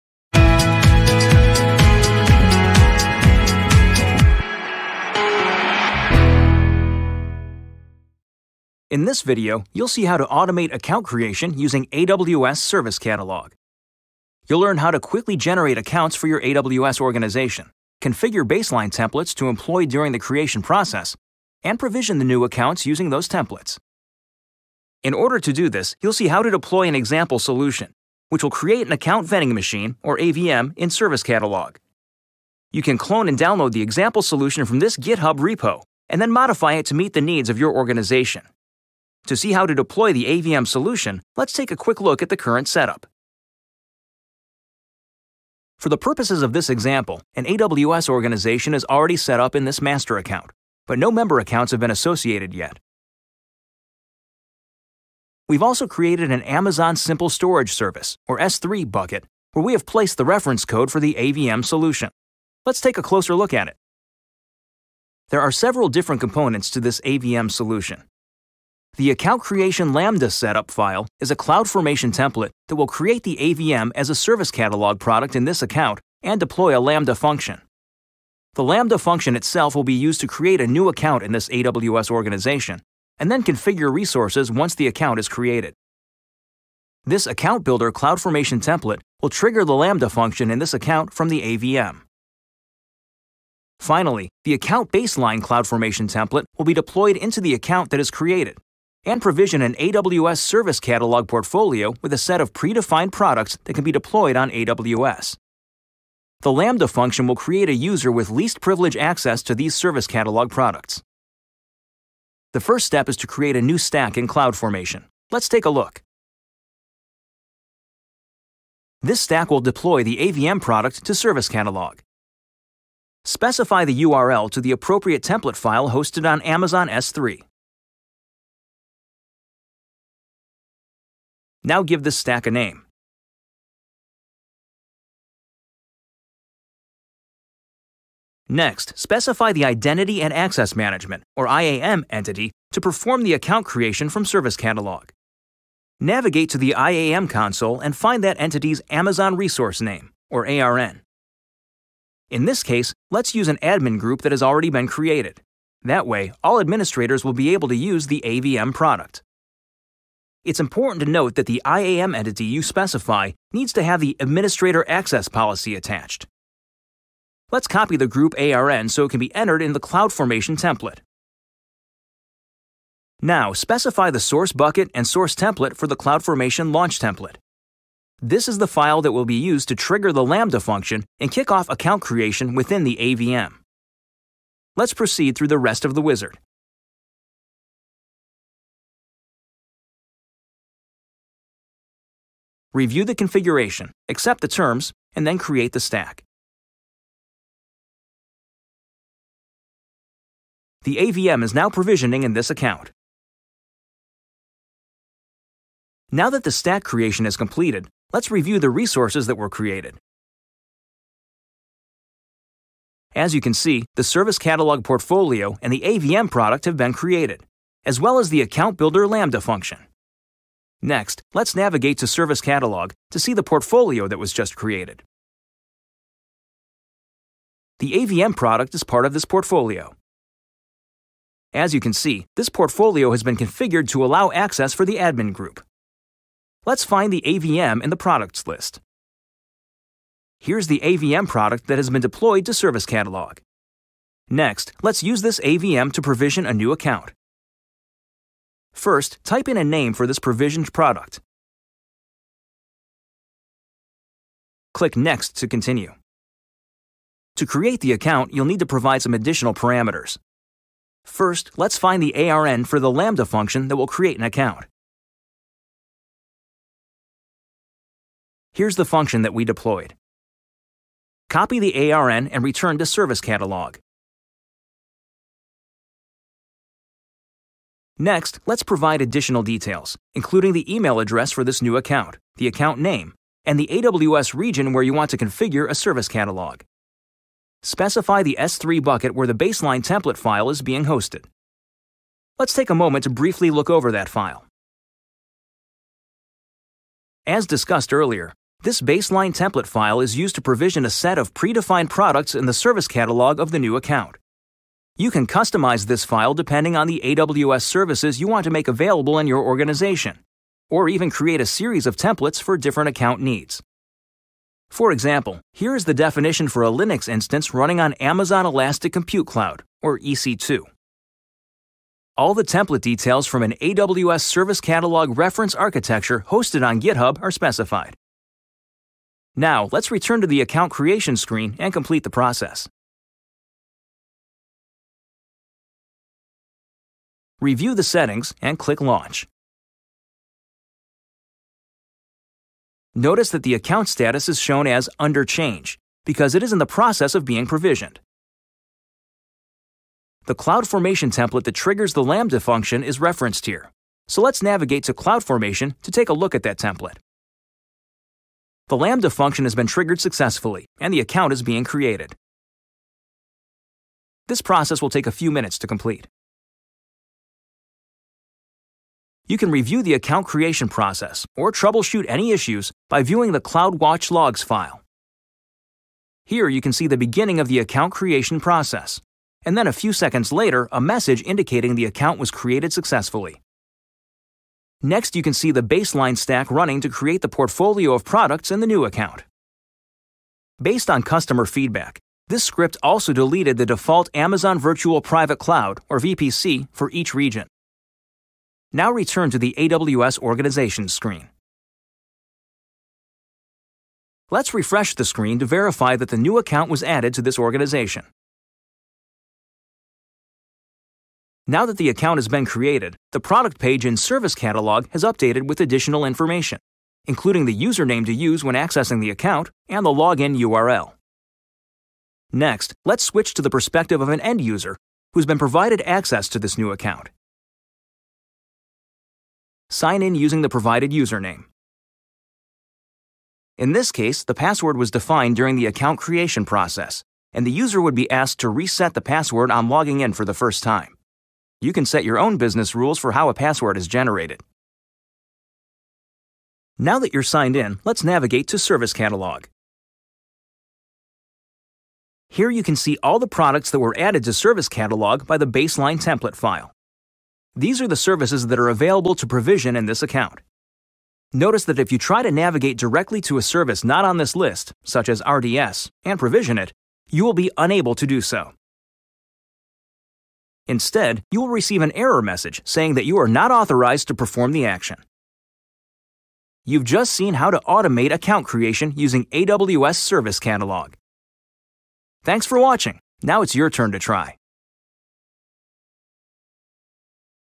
Automate Account Creation Using AWS Service Catalog
In this video, you’ll see how to automate account creation using AWS Service Catalog. You’ll learn how to quickly generate new accounts for your AWS Organization, configure baseline templates to employ during the creation process, and provision the new accounts using those templates.